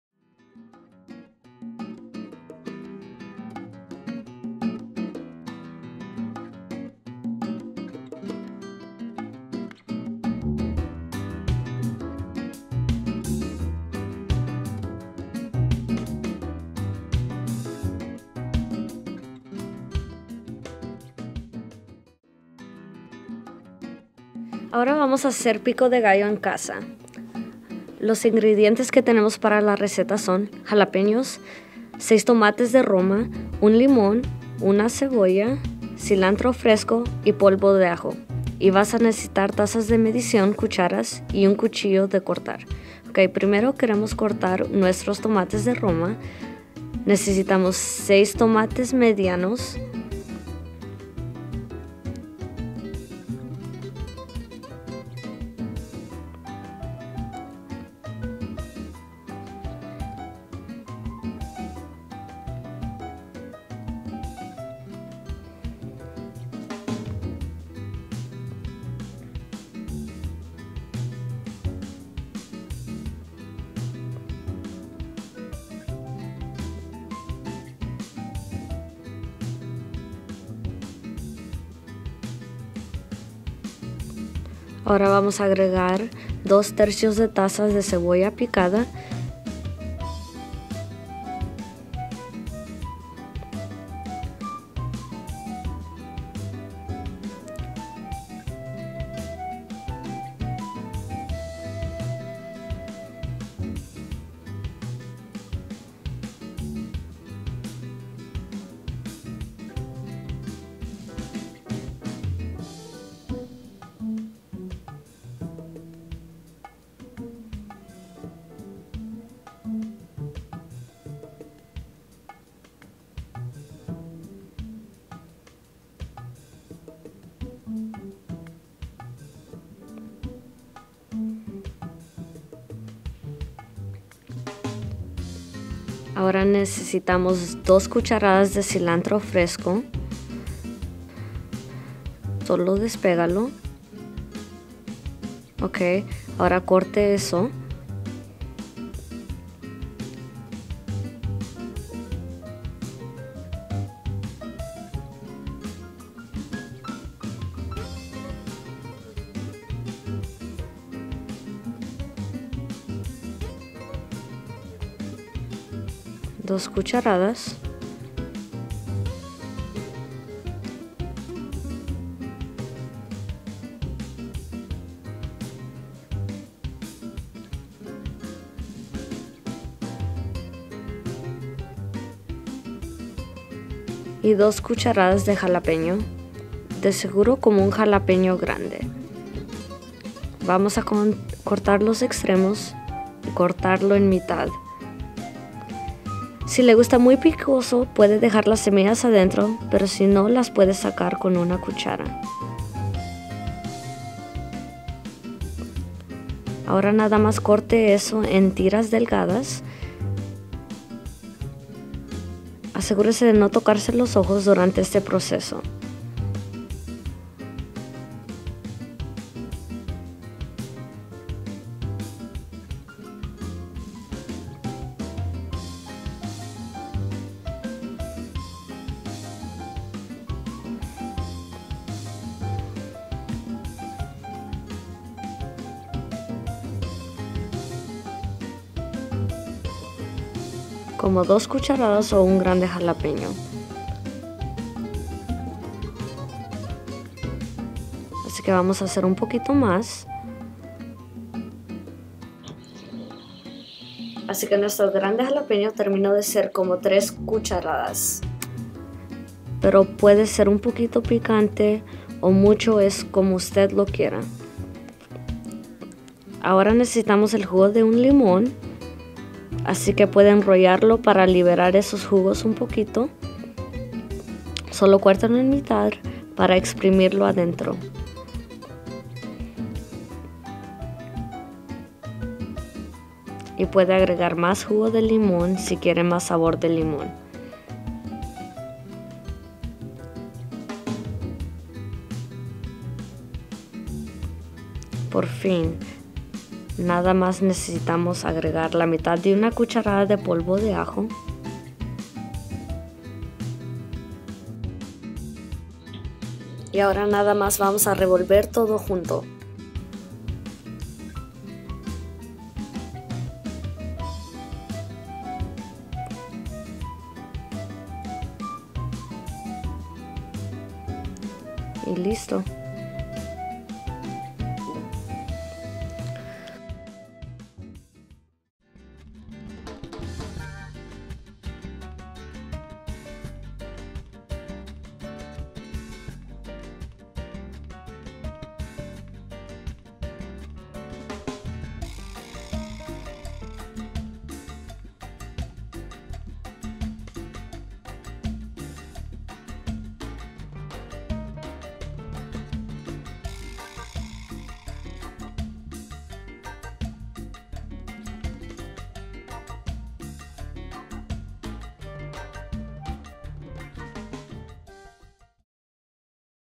Cuadro 1 Cocinando: Pico de Gallo Fresco
Ingredientes:

2 ½ tazas de Tomates Romanos
⅔ taza de Cebolla Picada
2 cucharadas de Cilantro Picado
2 cucharadas de Jalapeño Picado
2 cucharadas de Jugo de Limón Fresco
½ cucharadita de Polvo de Ajo